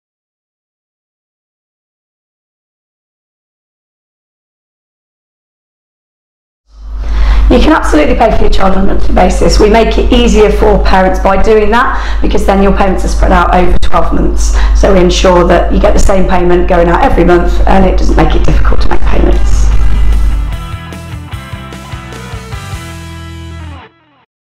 didi rugby FAQs - Can I pay for my child on a monthly basis?
FAQs: Can I pay for my child's didi rugby class on a monthly basis?

A: Yes you can.

Your first session is free, and after that didi rugby costs vary dependent on the area you live in and the franchise base you are attached to.

Those costs range from £19 up to £29 a month and can be settled via a monthly direct debit.